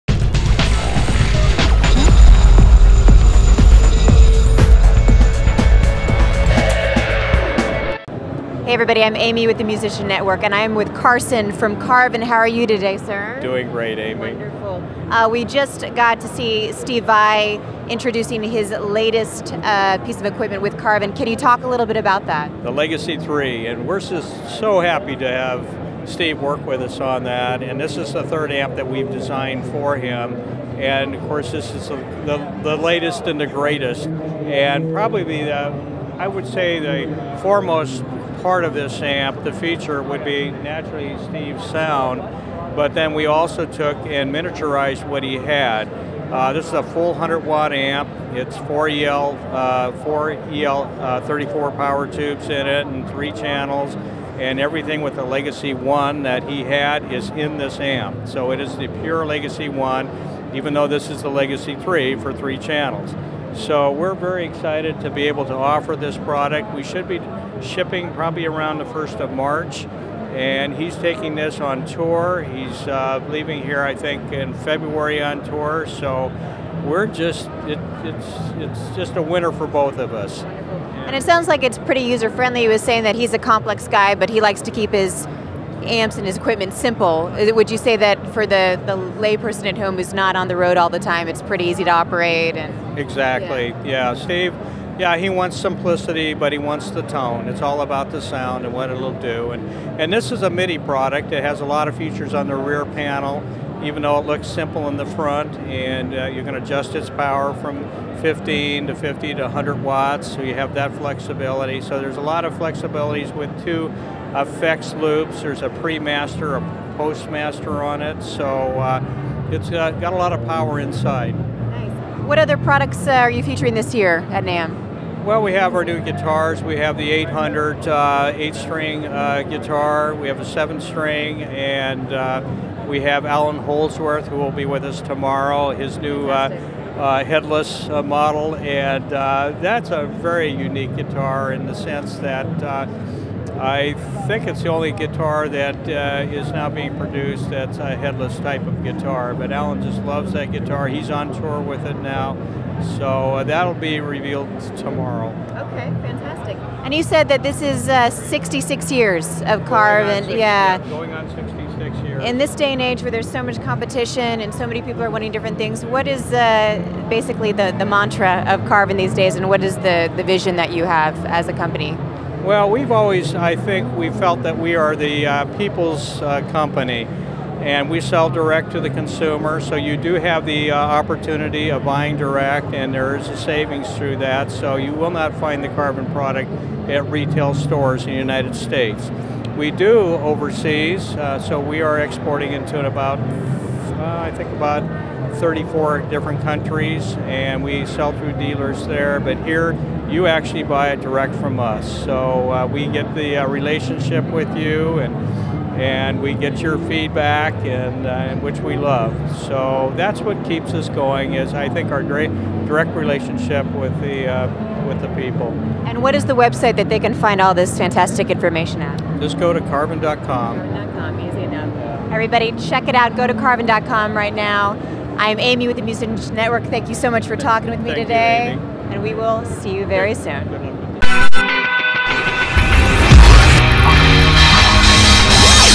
Carvin: NAMM 2012 Interview
Amy hits up the Carvin booth to see whats happening with this industry staple at the NAMM 2012 Show.